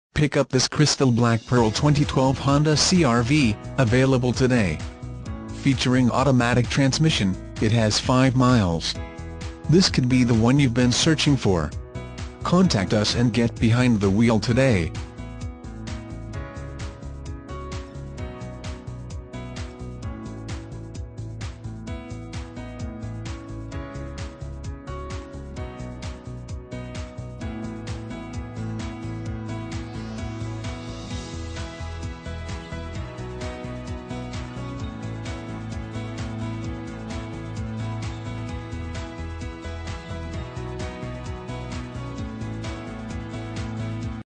2012 HONDA CR-V Huntington, WV H12396
2012 HONDA CR-V Huntington, WV
Stock

Moses Honda
5210 US Rt. 60 E.

Pick up this Crystal Black Pearl 2012 Honda CR-V, available today. Featuring automatic transmission, it has 18 miles. This could be the one you've been searching for.